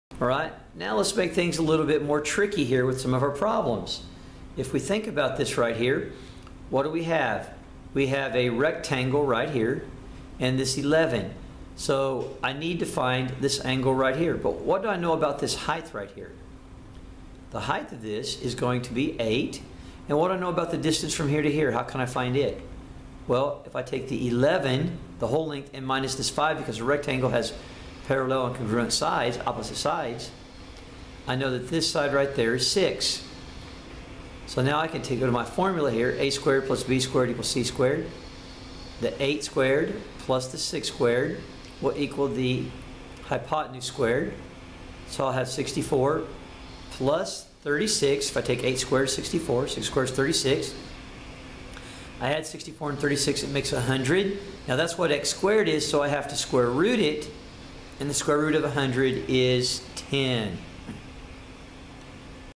Pythagorean Theorem using a rectangle
Using pythagorean theorem when the figure is not a triangle.  This example has a trapezoid with two 90 deg angles with three sides of 5, 8 and 11.